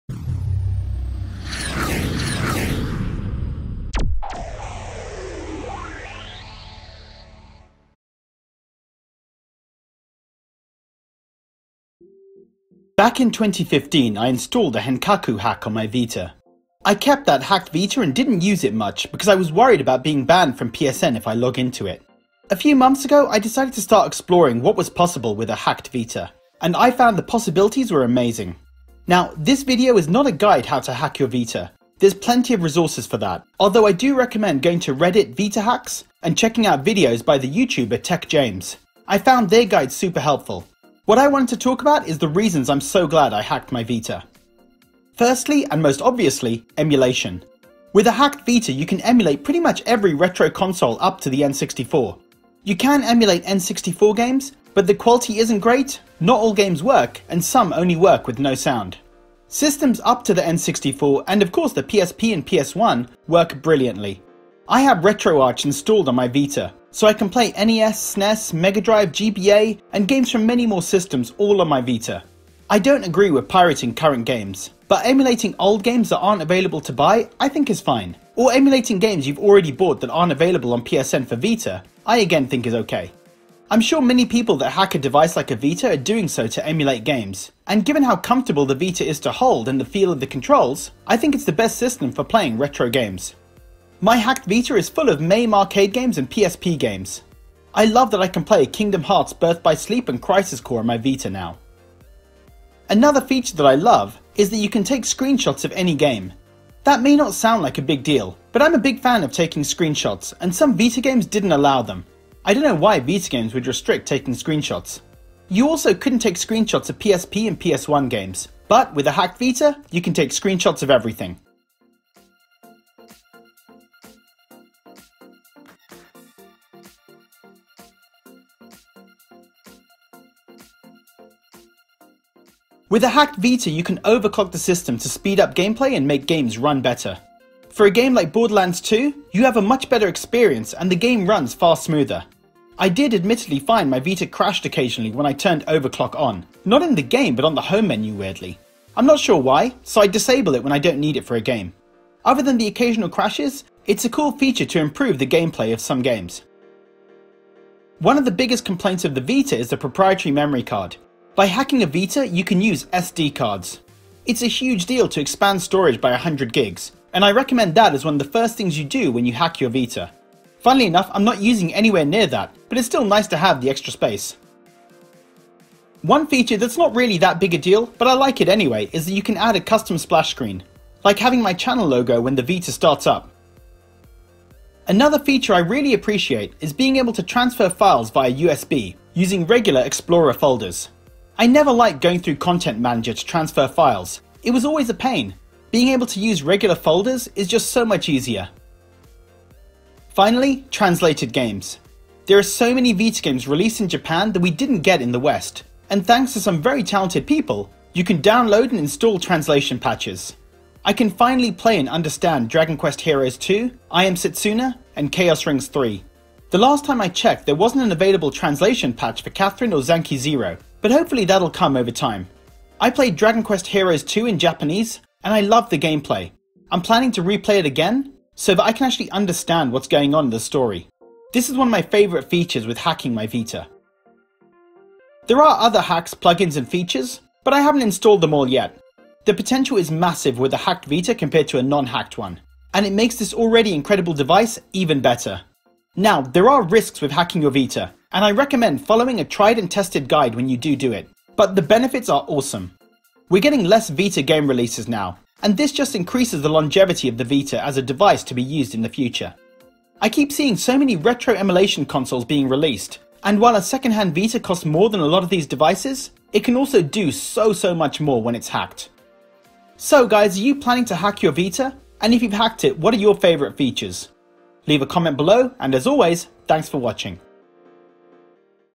I hacked my PS Vita, and I love it!
Hi guys, I hacked my PSVita back when the Henkaku jack was released but I never really used it or took advantage of it until recently, and I've discovered that a hacked PS Vita is just awesome. I recommend going to reddit vita hacks or watching videos from the YouTuber Tech James for guides on how to hack your Vita.